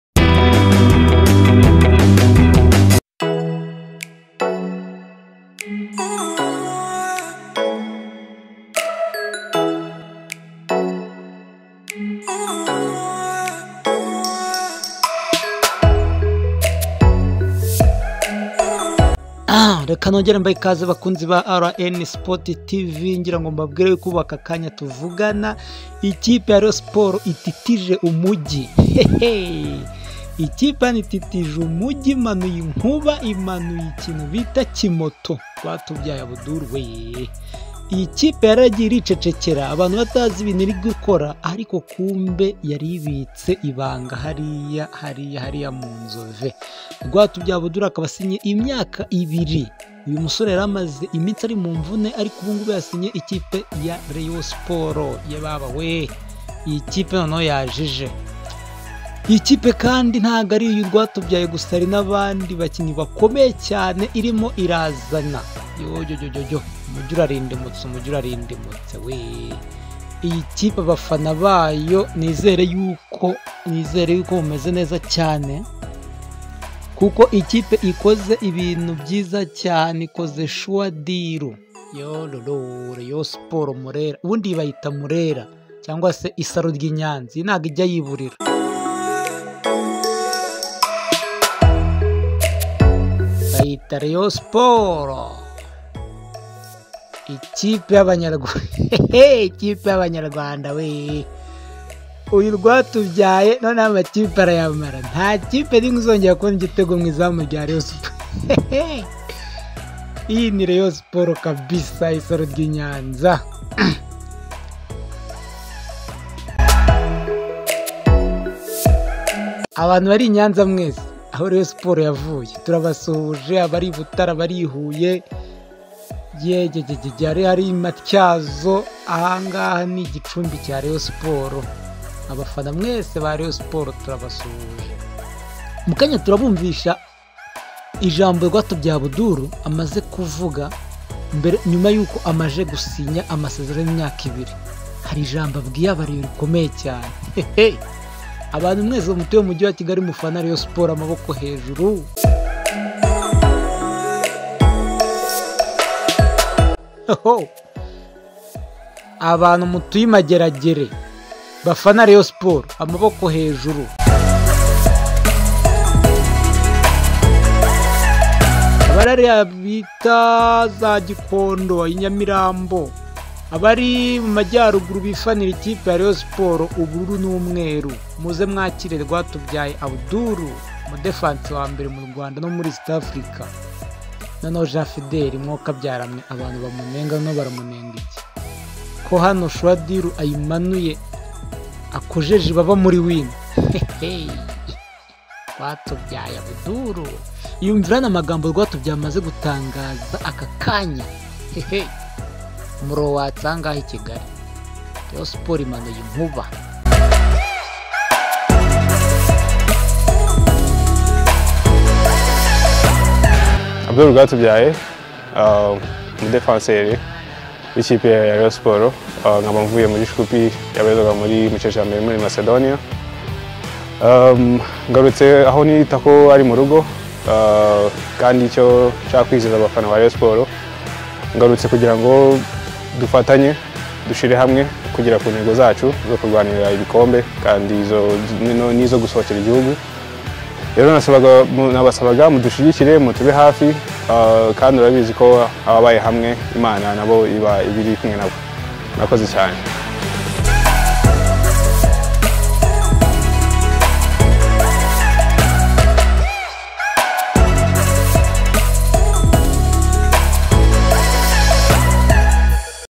RWATUBYAYE ABDUL AVUZE AMAGAMBO AKOMEYE NYUMA YO GUSINYIRA RAYON SPORTS || IYI KIPE ITITIJE KIGALI
Rwatubyaye Abdul ngo akeneye IGIKOMBE cya Shampiyona